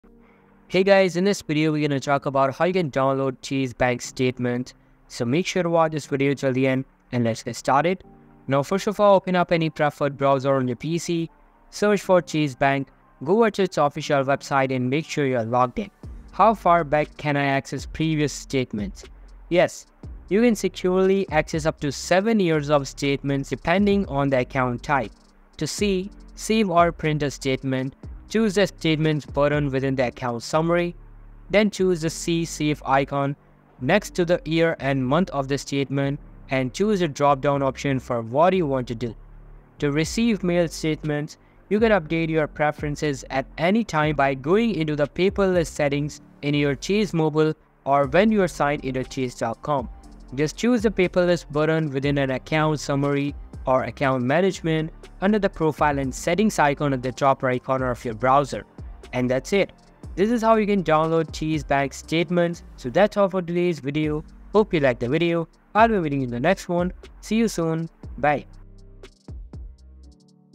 How To Download Chase Bank Statements (2025)
In this video I'll show you how to get chase bank statement pdf. The method is very simple and clearly described in the video. Follow all of the steps and see how to get chase bank statement.